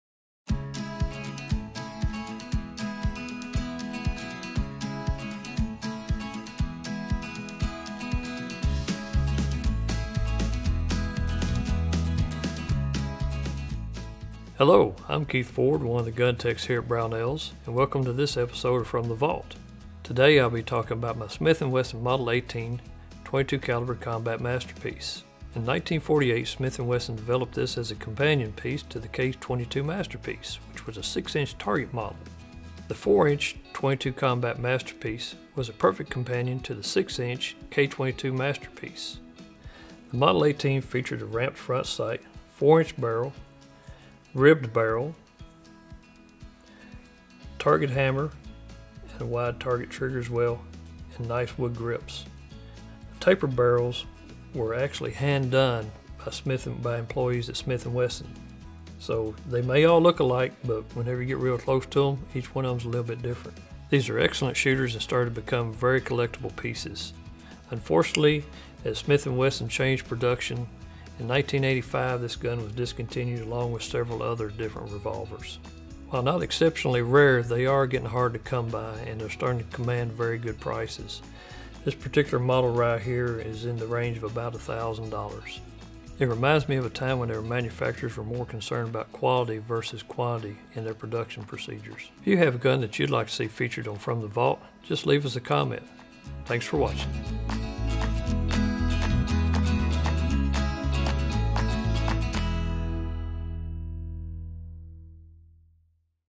From the Vault: Smith & Wesson Model 18 (22 Combat Masterpiece)
Join Brownells GunTech Keith Ford as he offers a quick look at the Smith & Wesson Model 18 (22 Combat Masterpiece). Built on the Smith & Wesson “K” Frame the Model 18 sports a tapered, 4-inch barrel, ramp front sight, target hammer, target trigger and is chambered in .22 LR. The Model 18 is an excellent shooter and much sought after among collectors. If you have a firearm you’d like to see featured in the From the Vault series, please let us know.